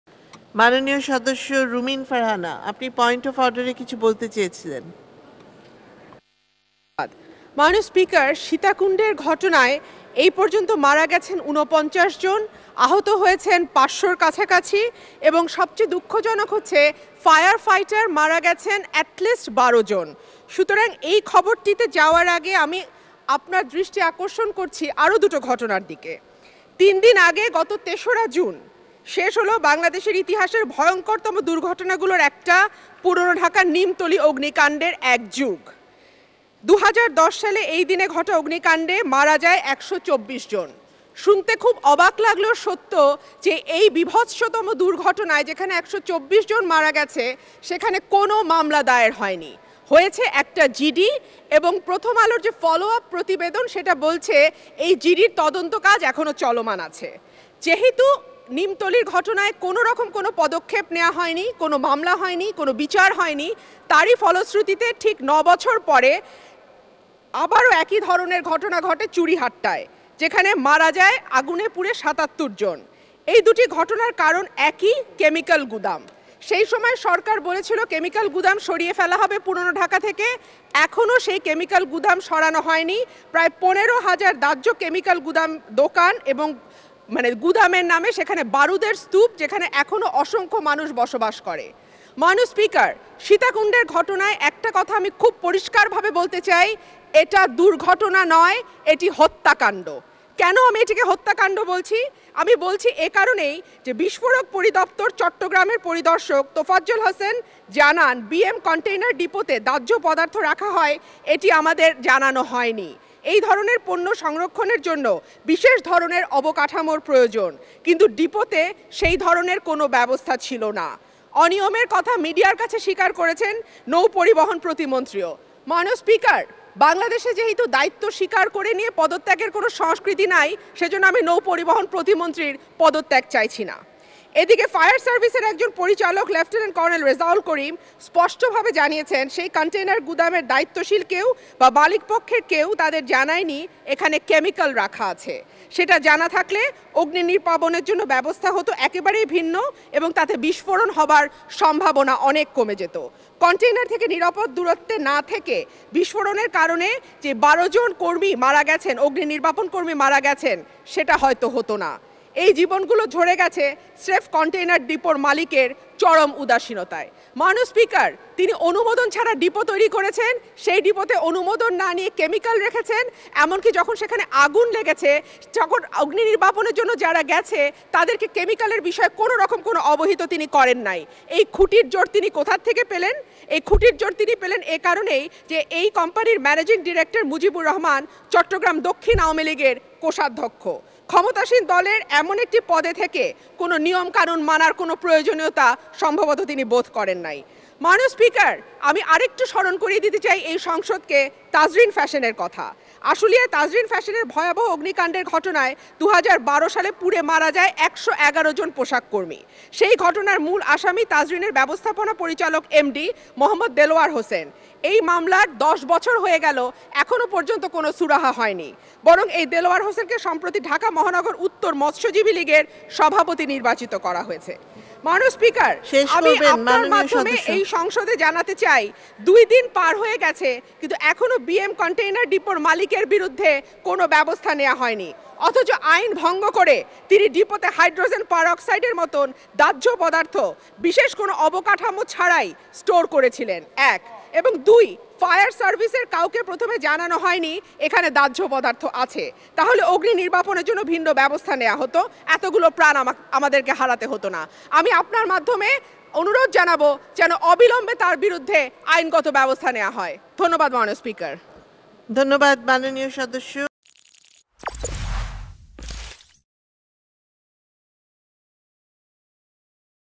সীতাকুণ্ডের ঘটনাকে ‘হত্যাকাণ্ড’ বললেন এমপি রুমিন | Rumeen Farhana | Sitakunda Tragedy | Firefighter
চট্টগ্রামের সীতাকুণ্ডের বিএম কনটেইনার ডিপোর অগ্নিকাণ্ডের ঘটনাকে হত্যাকাণ্ড বলে আখ্যায়িত করেছেন সংরক্ষিত নারী আসনের সংসদ সদস্য ব্যারিস্টার রুমিন ফারহানা।